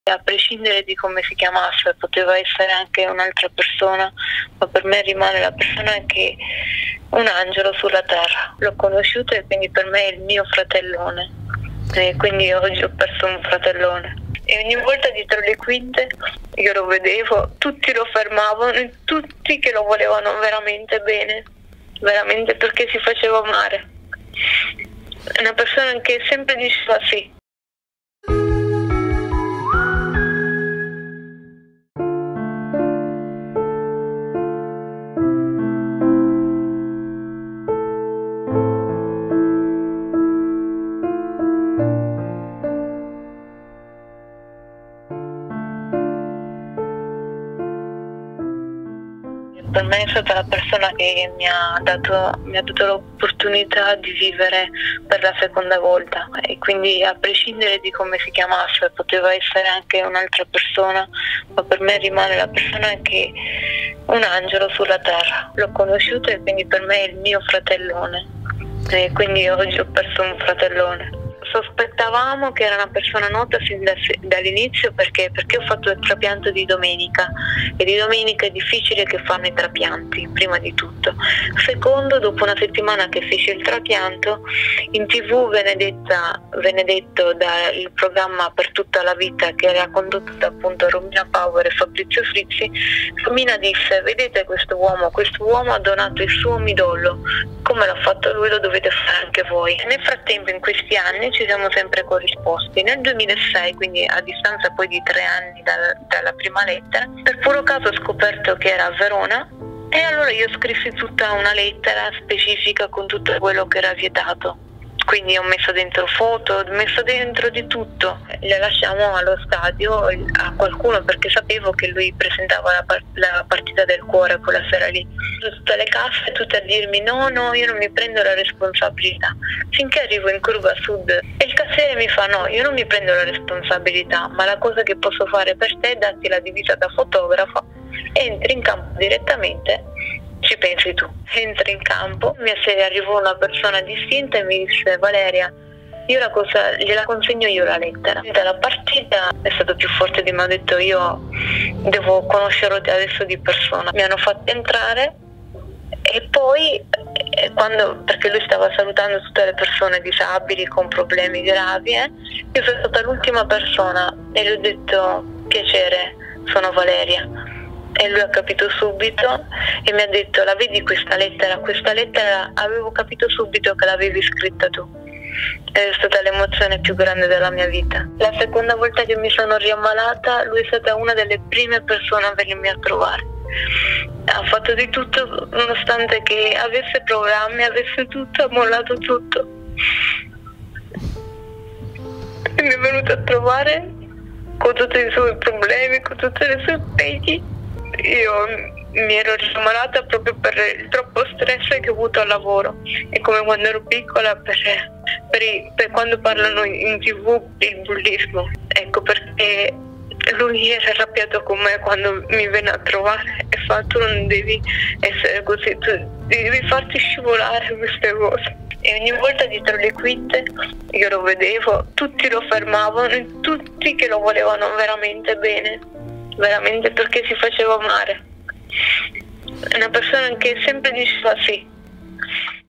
"Fabrizio Frizzi mi salvò la vita donando il midollo, un fratellone": il ricordo di Valeria Favorito
Nel 2000 Fabrizio Frizzi donò il midollo in forma anonima a Valeria Favorito, all’epoca 11enne e malata di leucemia, salvandole la vita. Dopo anni la ragazza ha scoperto che il suo donatore era il famoso conduttore e, dopo averlo rintracciato, sono diventati inseparabili. Oggi Valeria ha 30 anni e ricorda con affetto il suo salvatore: “Caro fratellone, se adesso sono qui a scrivere è proprio grazie a te, il sangue che mi scorre tra le vene è il tuo”.